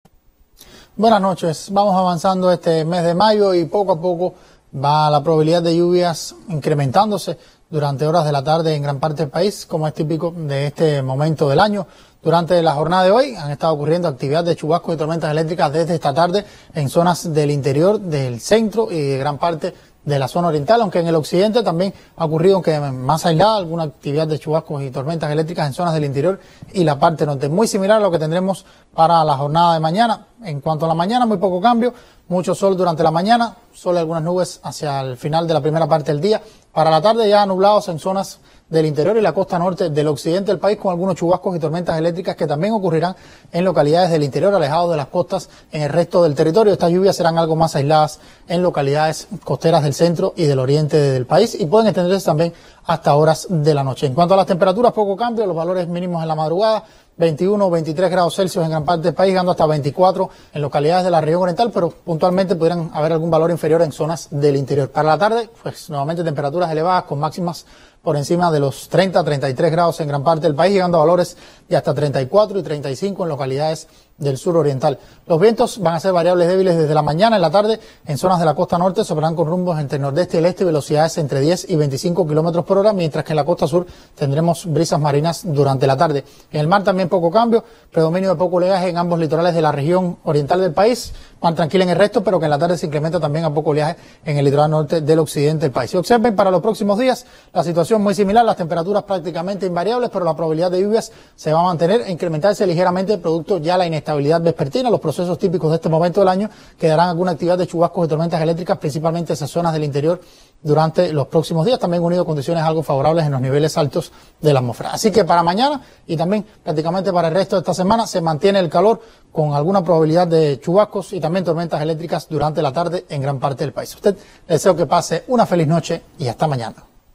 Actualización meteorológica de Cuba
Sobre las variables meteorológicas en Cuba para este martes 9 de mayo de 2023 y en los próximos días, ofrecemos detalles en nuestra emisión Estelar.